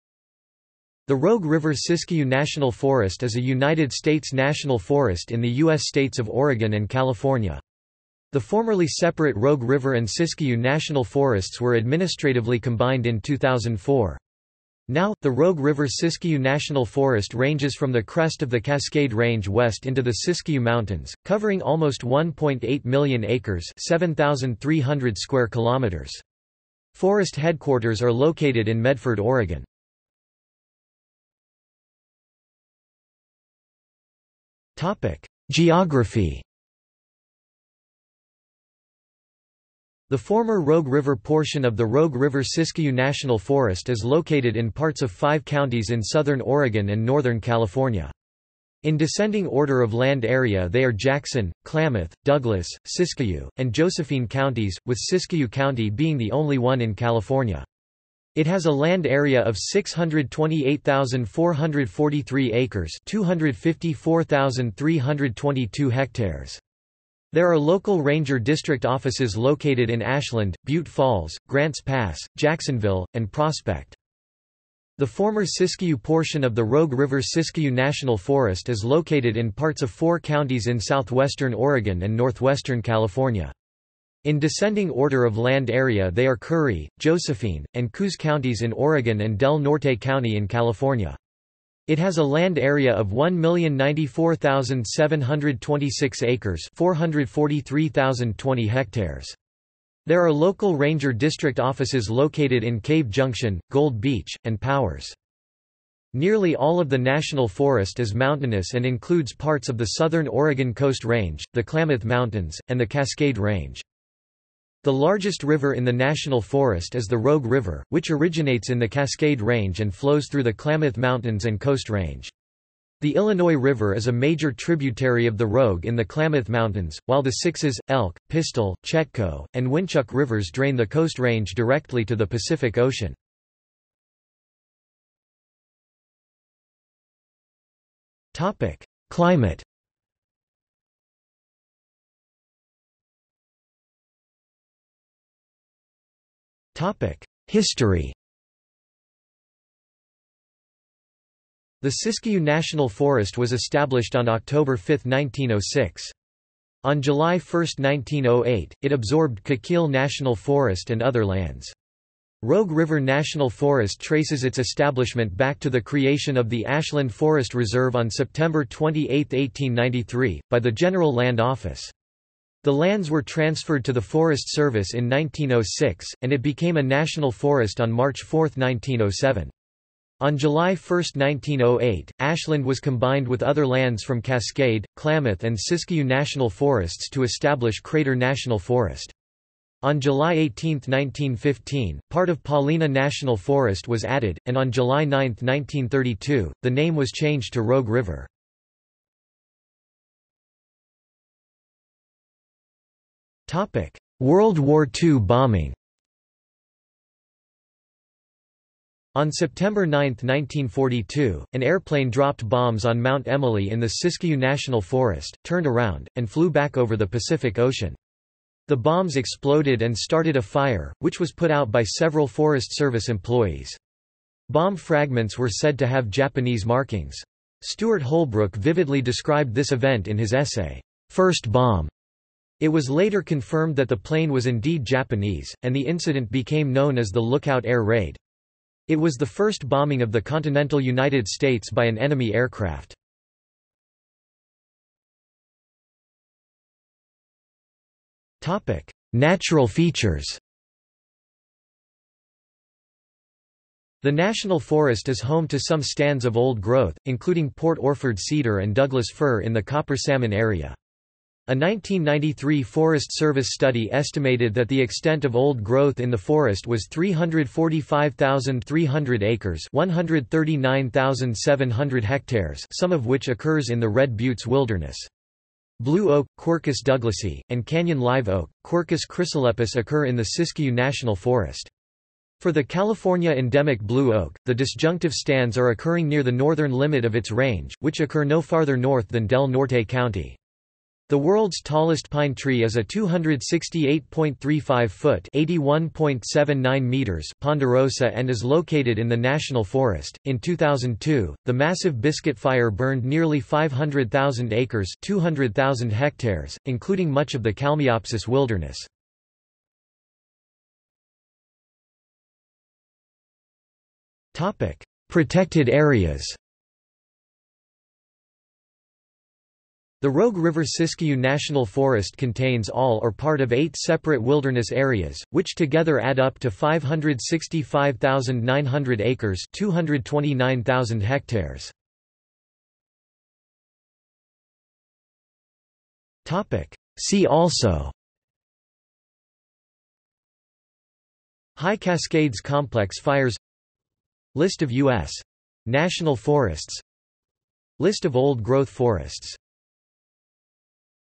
Rogue River – Siskiyou National Forest | Wikipedia audio article
This is an audio version of the Wikipedia Article:
Rogue River – Siskiyou National Forest

00:00:31 1 Geography
00:02:15 1.1 Climate
00:02:23 2 History
00:03:20 2.1 World War II bombing
00:04:04 3 Natural features
00:05:10 4 Protected areas
00:05:32 5 See also

- Socrates

SUMMARY
The Rogue River–Siskiyou National Forest is a United States National Forest in the U.S. states of Oregon and California.  The formerly separate Rogue River and Siskiyou National Forests were administratively combined in 2004.  Now, the Rogue River–Siskiyou National Forest ranges from the crest of the Cascade Range west into the Siskiyou Mountains, covering almost 1.8 million acres (7,300 km2). Forest headquarters are located in Medford, Oregon.